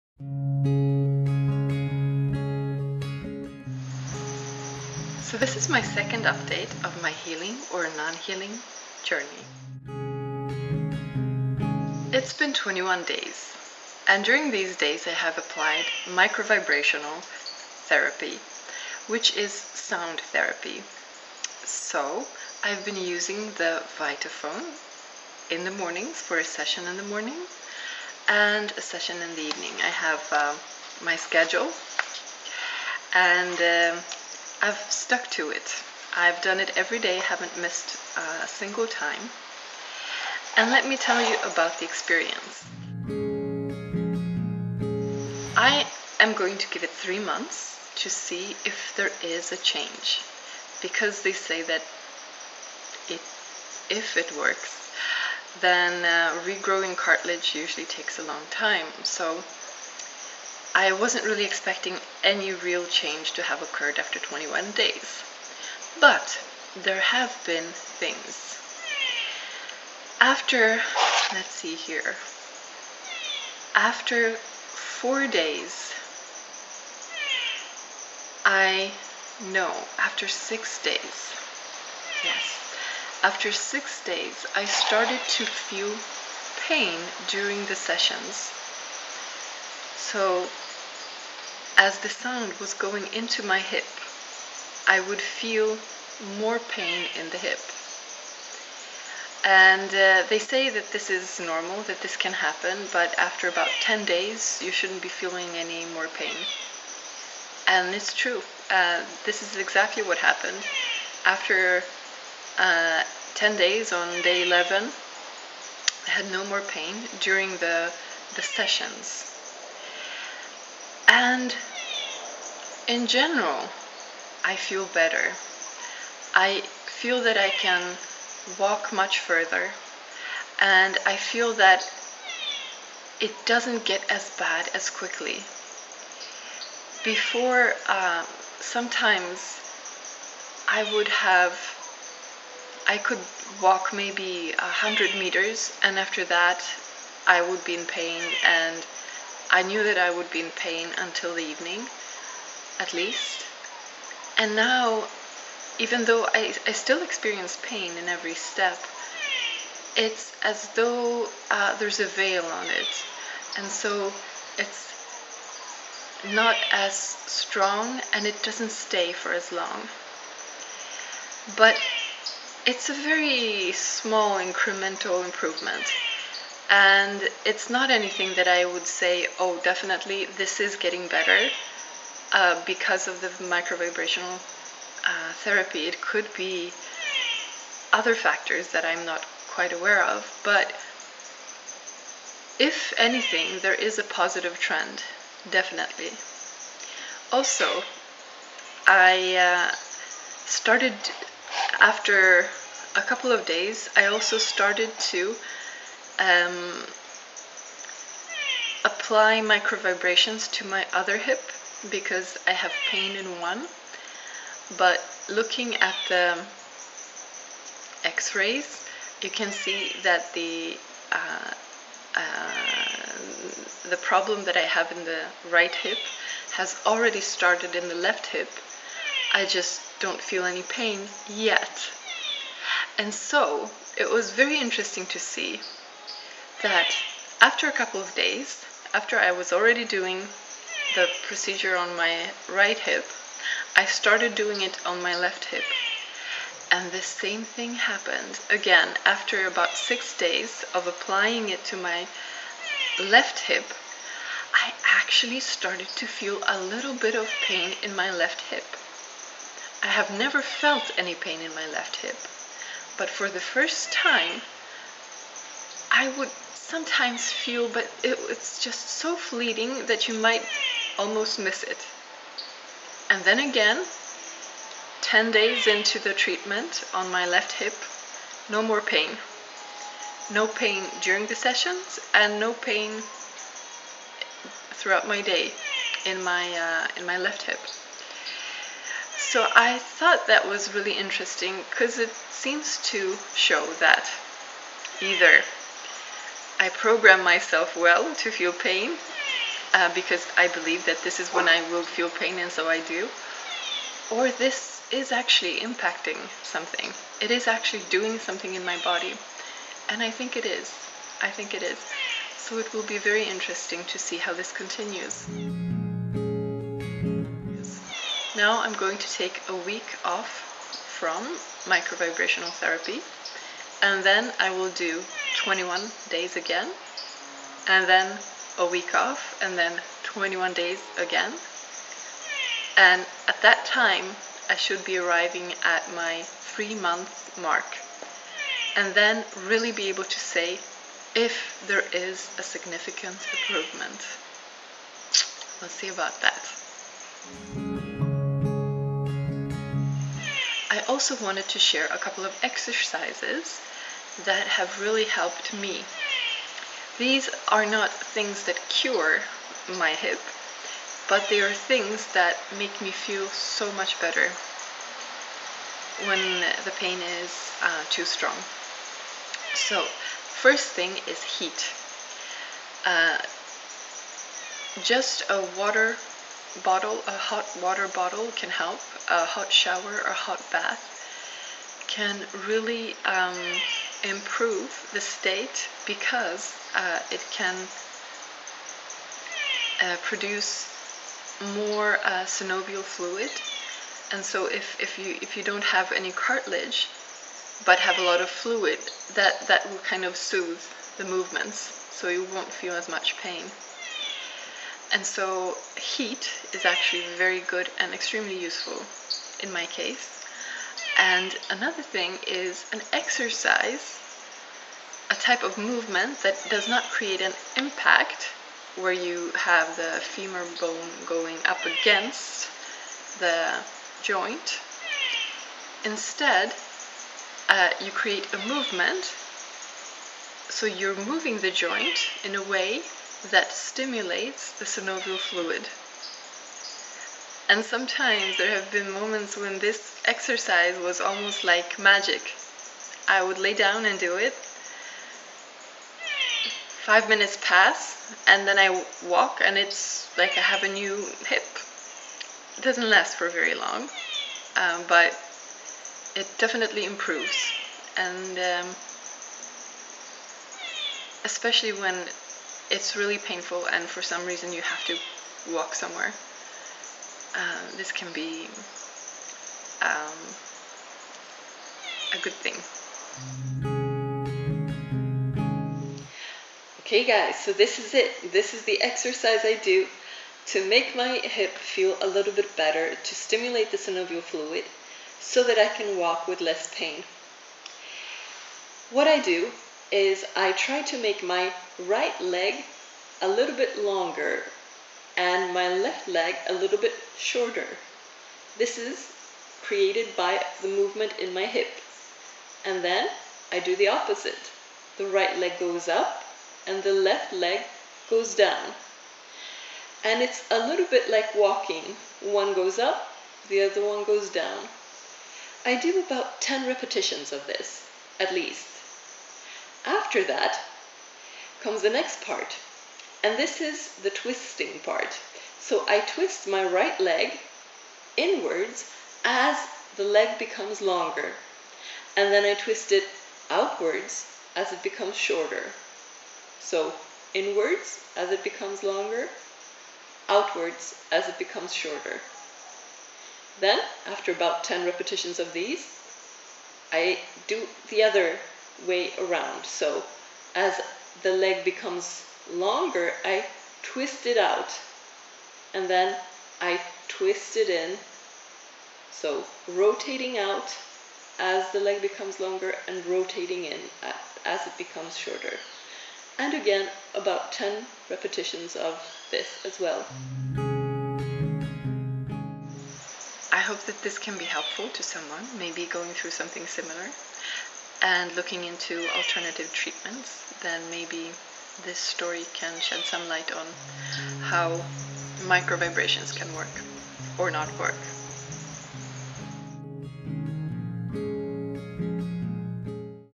My Journey with Osteoarthrisis Part 2
This is my healing/non-healing journey of osteoarthritis.  I do not know the outcome of the journey.  My mode of healing:  micro vibrations.  This update is after one month of treatment.  I plan on doing 6 months.

My dad gave me a Vitafon for my birthday, and so I am using it to see if I can heal from something mainstream medicine tells me is incurable and only will get worse.

This may prove to be a rather long journey, as regrowing cartilage is not a quick thing to do.  Cartilage has no blood flowing to it to bring it nutrients.  Instead it is only nourished through synovial fluid.  And so, my miracle will surely not be an immanent one, but may take its time, if it decides to happen at all.

Also, just because I just finished writing it, you should get my book and let me know what you think of it: